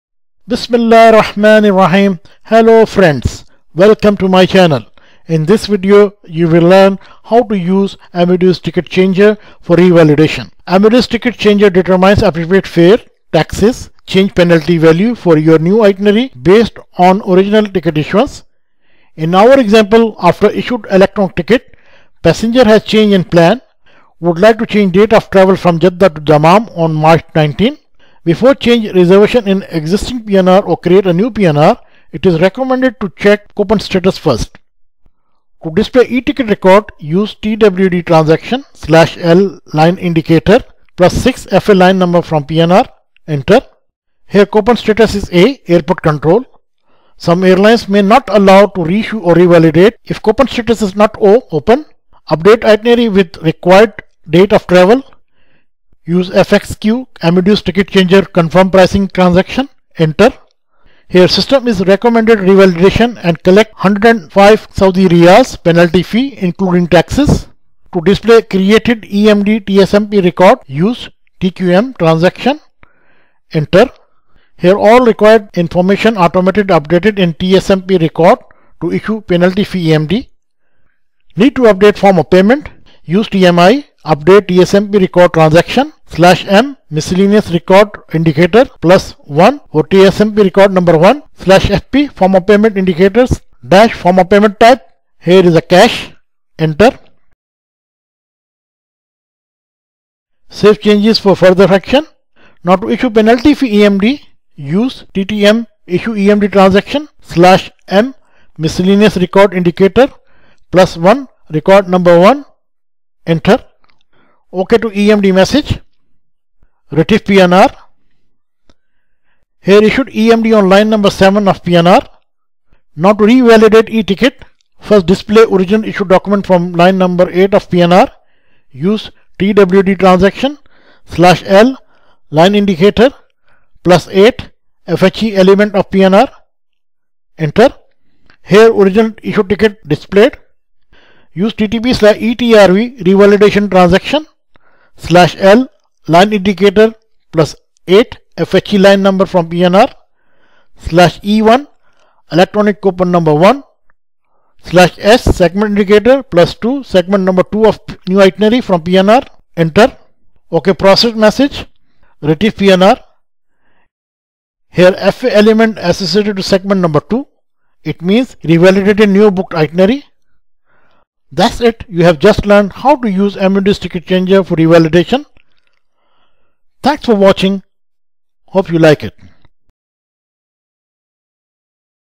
How to Revalidate a Ticket in Amadeus Using ATC – Quick & Easy!
Looking to revalidate a ticket quickly and efficiently using Amadeus Ticket Changer (ATC)? This in-depth tutorial will walk you through the entire revalidation process in Amadeus, ensuring you can make changes to a ticket without reissuing or additional fare collection.

Amadeus Ticket Changer (ATC) Revalidation is a powerful tool that allows travel agents and airline professionals to modify a passenger’s ticket details while keeping the original fare conditions intact. This process ensures that minor changes, such as flight time modifications or minor itinerary adjustments, are updated without requiring a full reissue.

✅ What is ATC Revalidation & How It Differs from Reissue
✅ Step-by-step guide to revalidate a ticket using Amadeus commands
✅ When to use revalidation instead of reissue
✅ How to verify ticket eligibility for ATC revalidation
✅ How to handle common errors during the process
✅ Tips for ensuring a seamless ticket modification experience

💡 Why Revalidate Instead of Reissue?

No Additional Collection (NOADC) – Saves time and avoids unnecessary fare adjustments.

Faster Processing – ATC automates the changes, reducing manual errors.

Maintains Ticket Conditions – Ensures compliance with airline ticketing rules without affecting fare rules.

Cost-Effective Solution – Reduces the need for costly fare recalculations.

✔️ Travel Agents Handling Ticket Modifications
✔️ Amadeus GDS Beginners & Advanced Users
✔️ Corporate Travel Managers Processing Changes
✔️ Airline Reservation Officers Managing Bookings

🔹 Watch till the end for expert tips on avoiding revalidation mistakes!